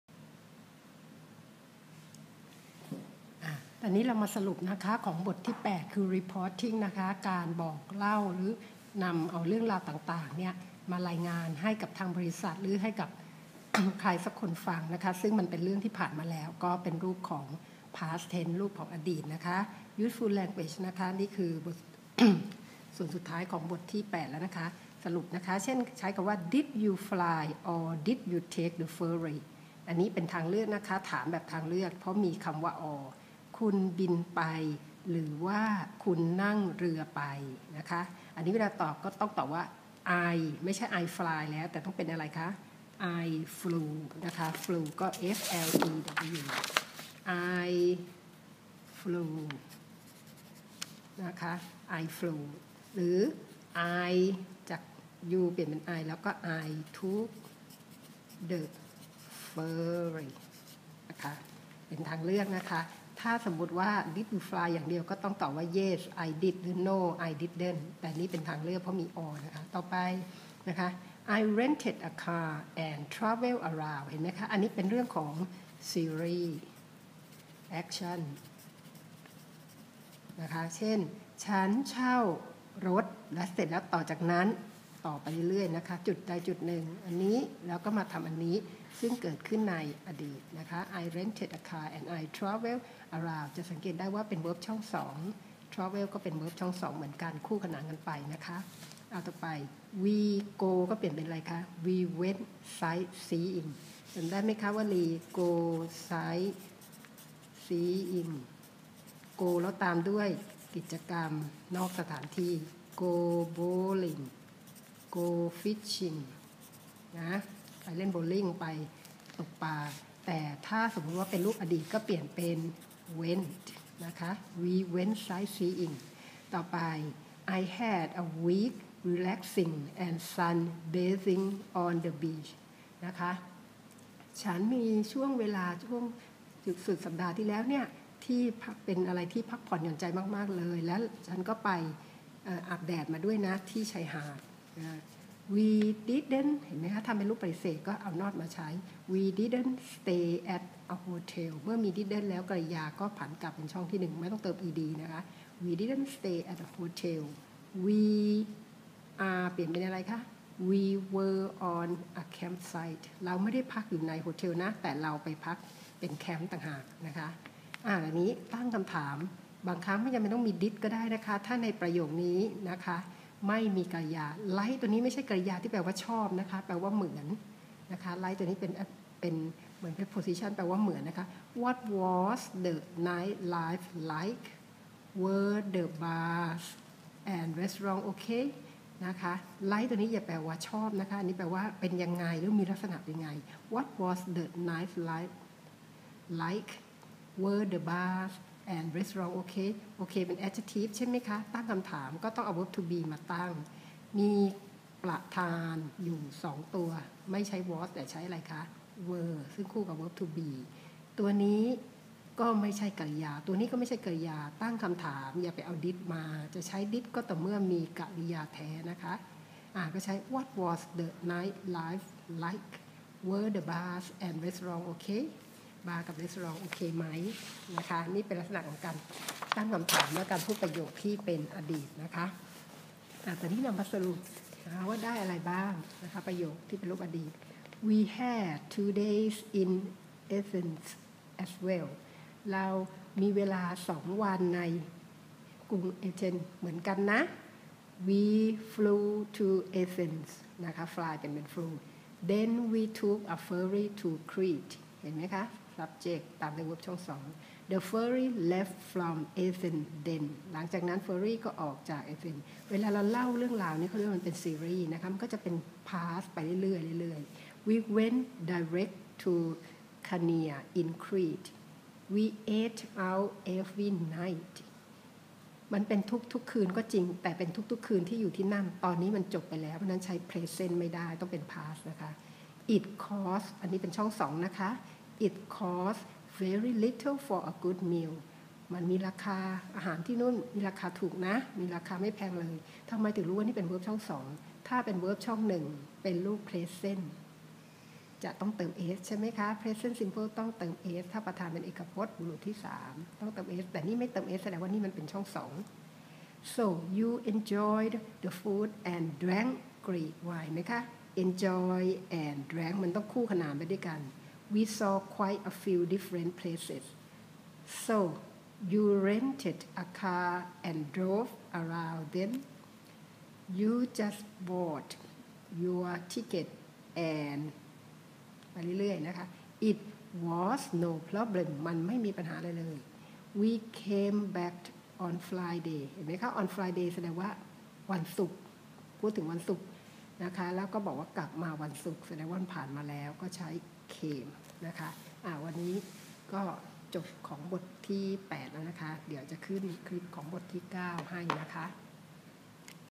3 U 8 สรุป Expressions บางช่วงเอามาจากคู่มือครู ต้องจดเอง
Start-Up 1: U.8 Reporting
U.8: สรุป Expressions about past events
(บางช่วงไม่มีในหนังสือเรียน ต้องจดเอง)